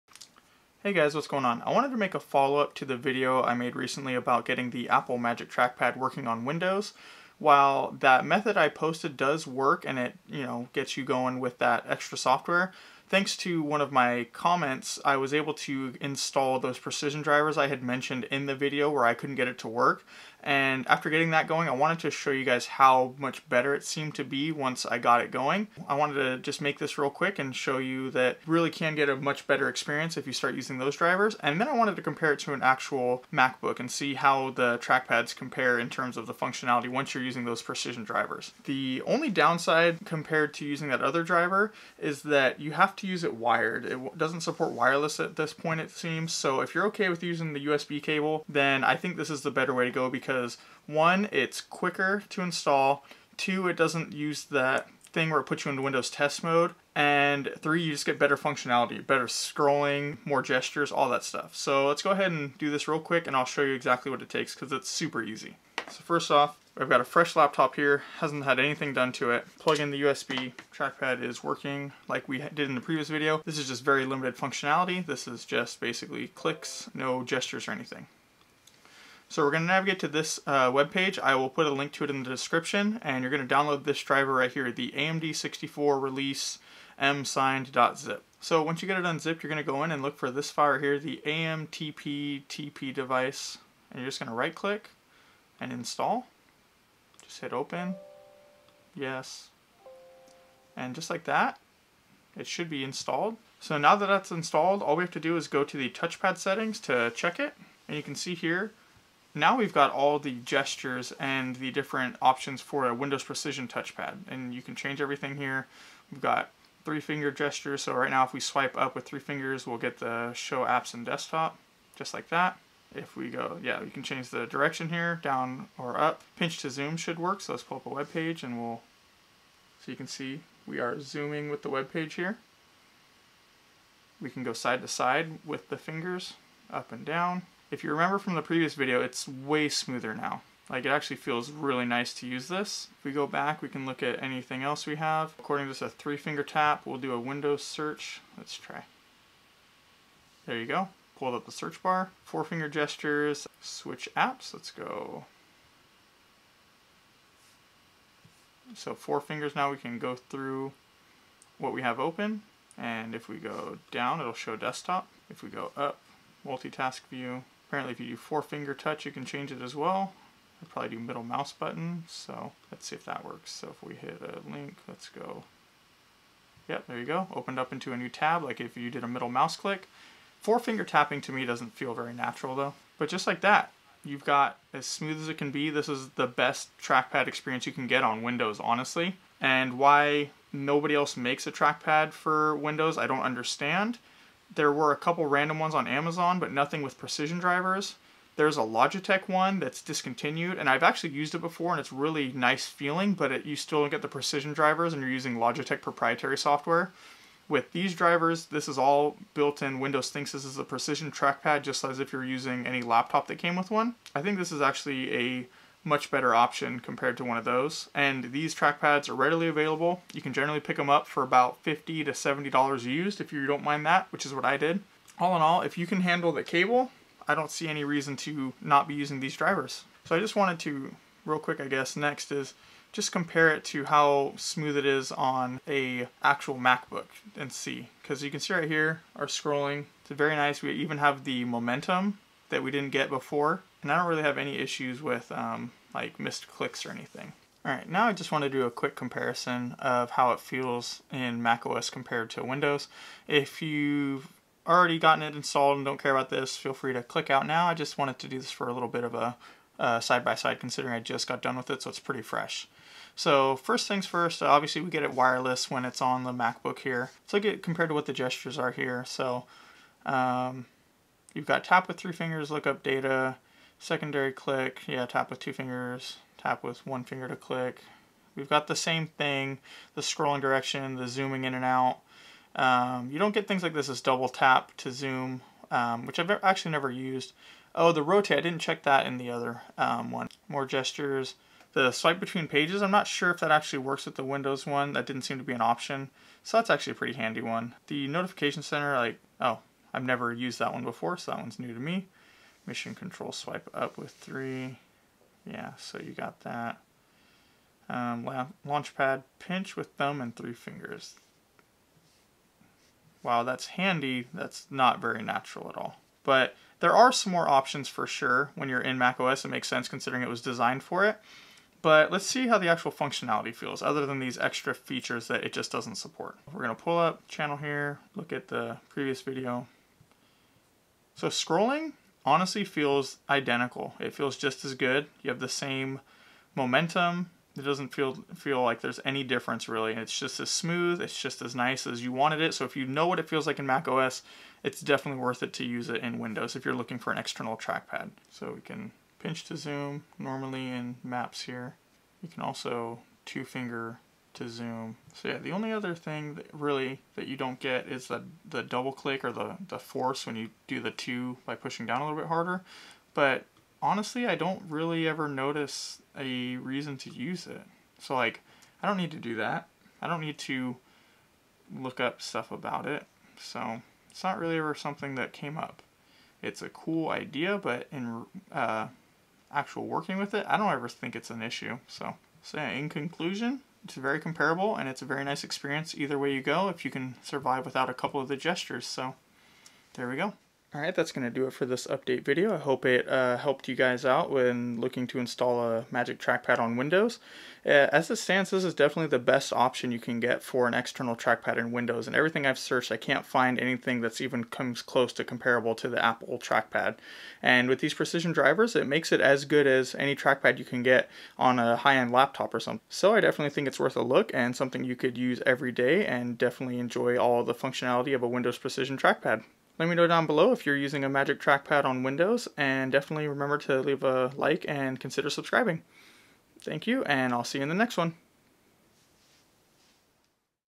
Install Apple Magic Trackpad with Windows Precision drivers (AS GOOD AS MacOS!)
This is a followup video to getting your Magic Trackpad 2 working in Windows 10. This method is not only simpler, but gives better results with full Windows Precision driver support!